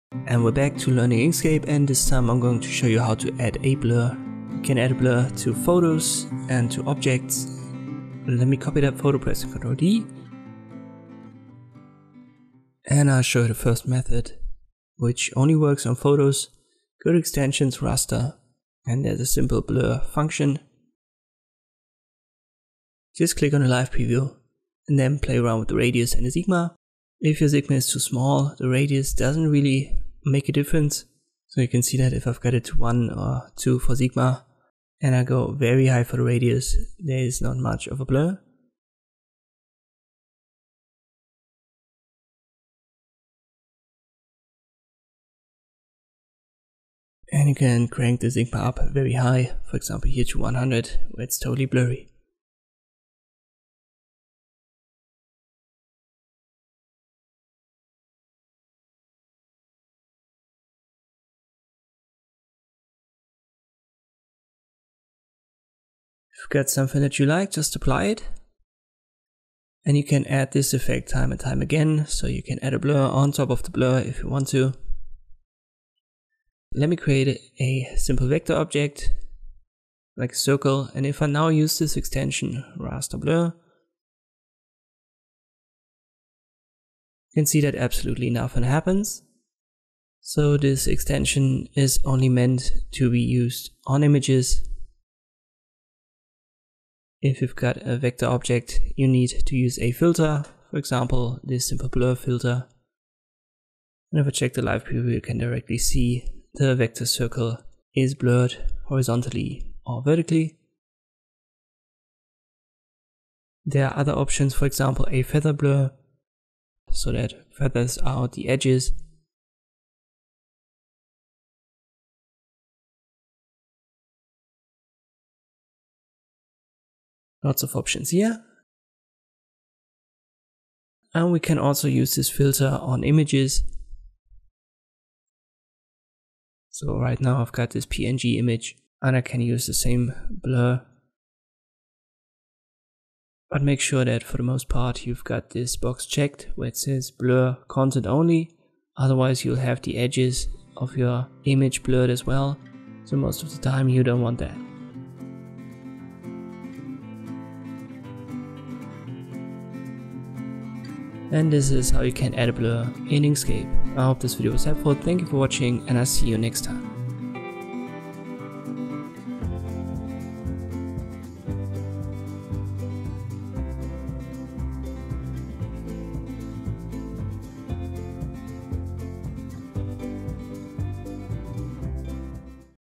How to add a blur in Inkscape
In this tutorial I'll show you how to add a Blur to images and vector elements in Inkscape.